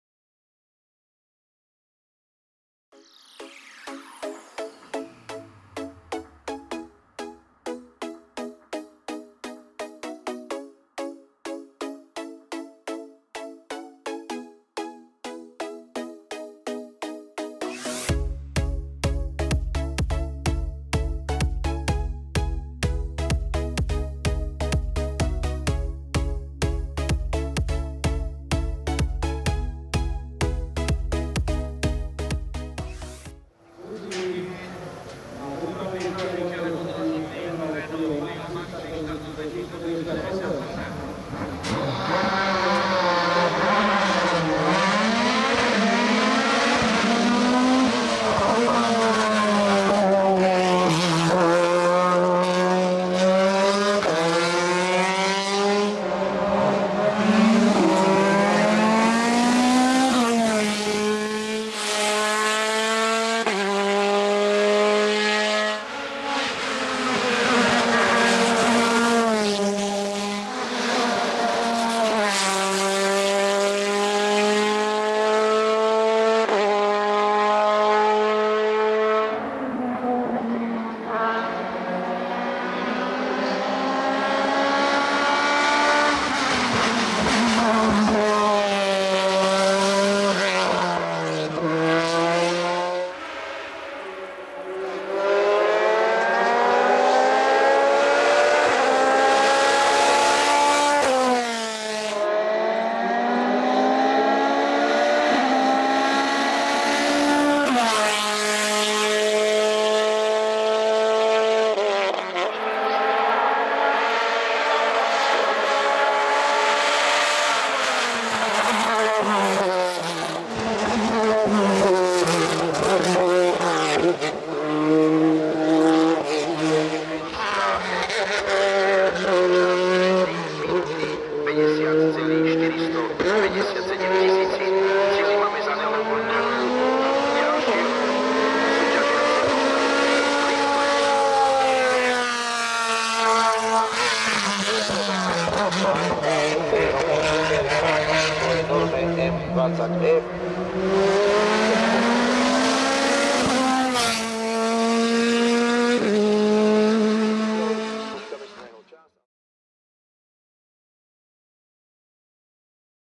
Michal Novický - Ligier JS49 Honda - XXXV. Dobšinský kopec 2018 - FIA European Hill Climb Champions
Michal Novický si na stroji Ligier JS49 Honda dojel ve třídě E2H-SC-2000 pro 2.místo na Dobšinském kopci 2018 a absolutně v České trofeji skončil na místě třetím.